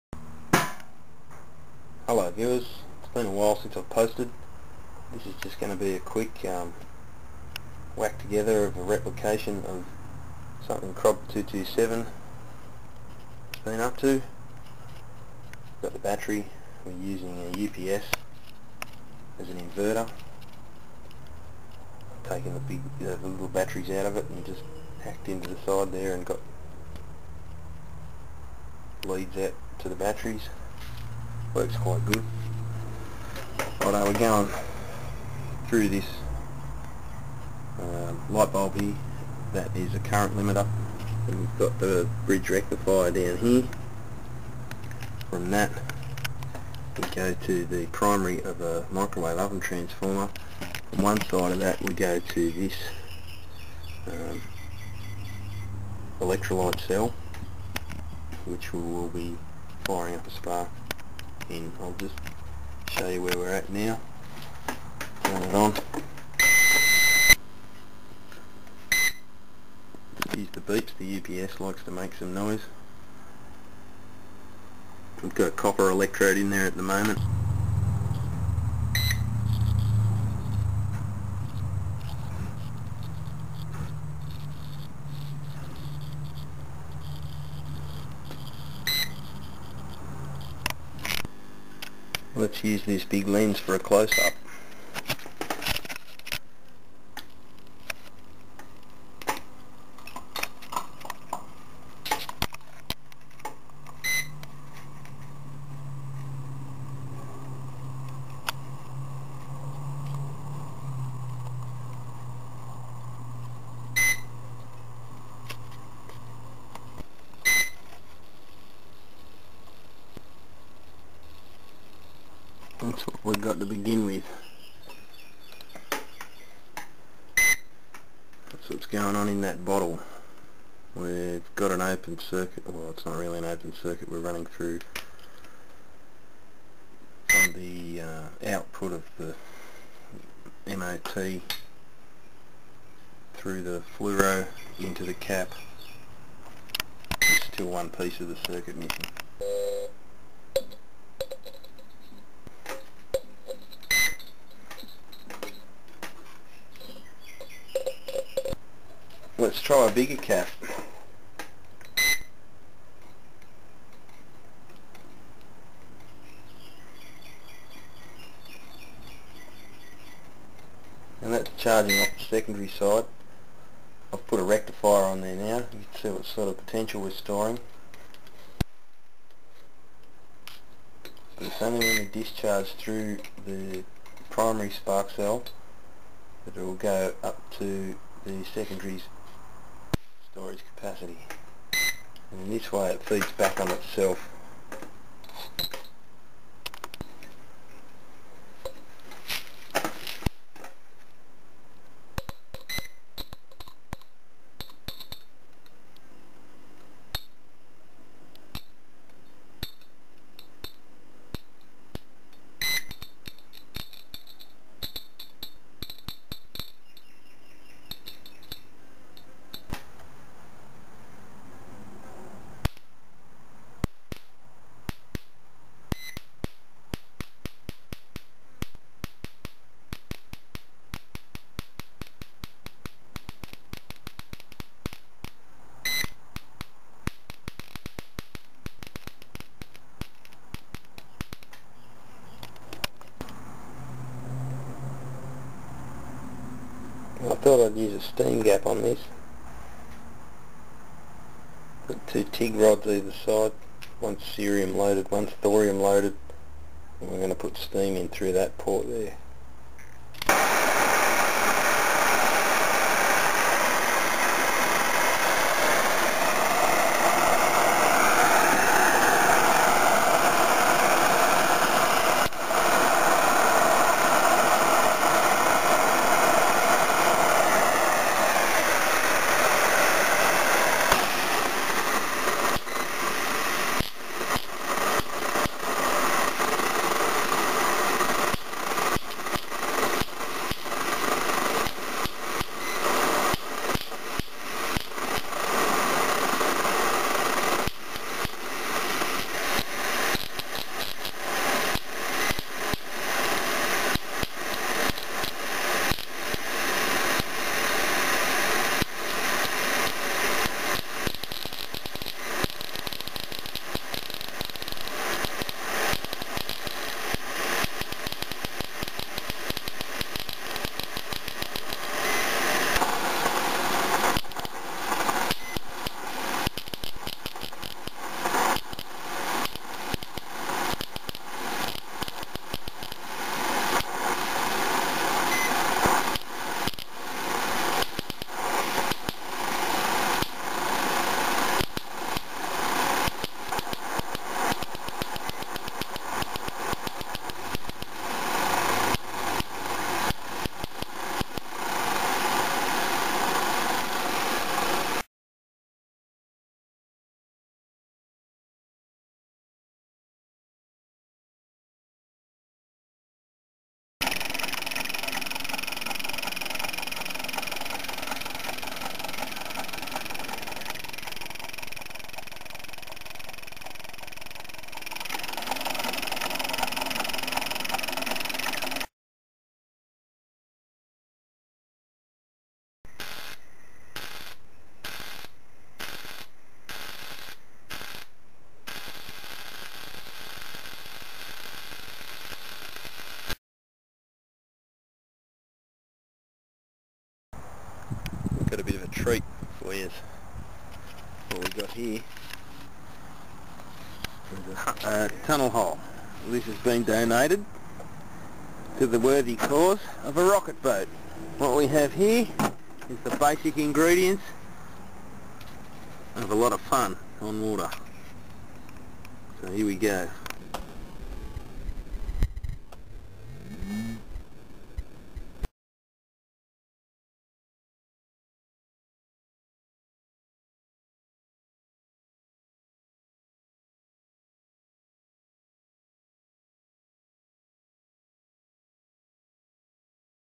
Steam HV Spark Gap 4+ extras
replication of crob227's resonance steam cell,and teaser for upcoming videos.
electrogravitcs,electric magnet motor,rocket boat.